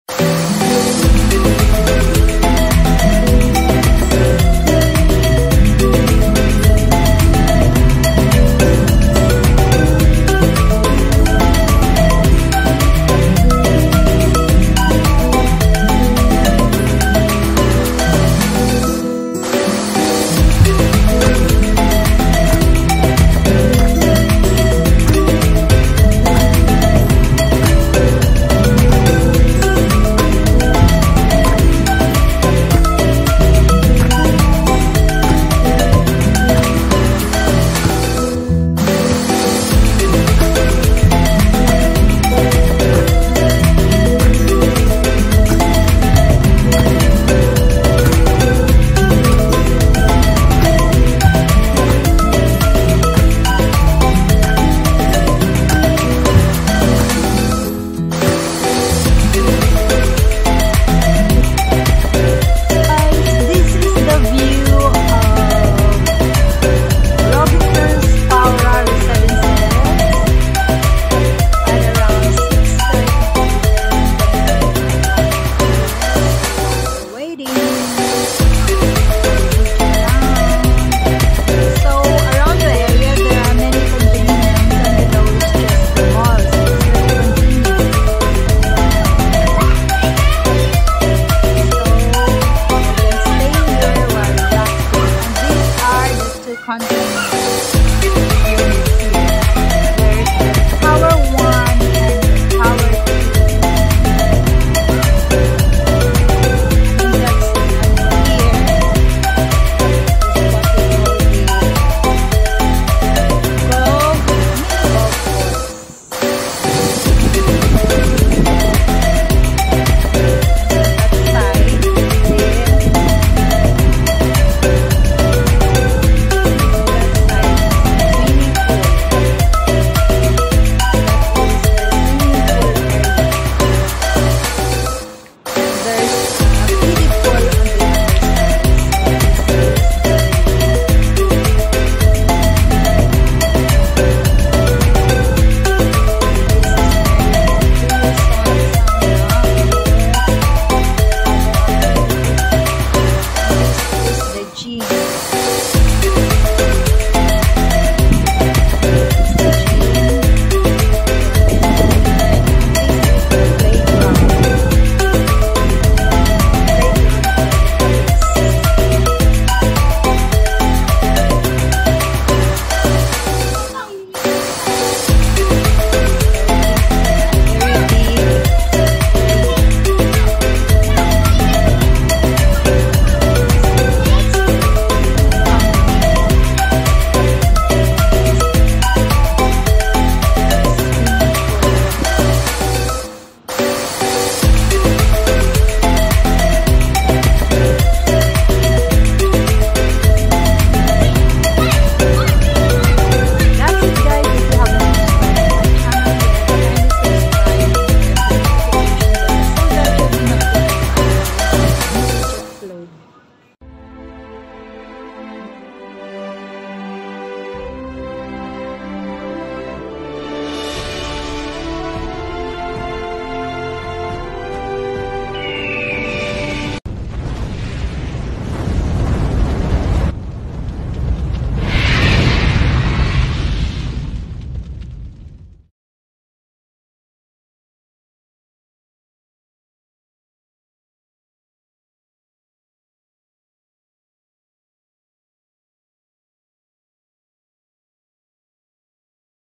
DAY TIME AND NIGHT TIME WALK TOUR - ROBINSONS PLACE WHERE I MEET MY CLIENT!
Having a day time and night tour of RPR Amenity Area and meeting my client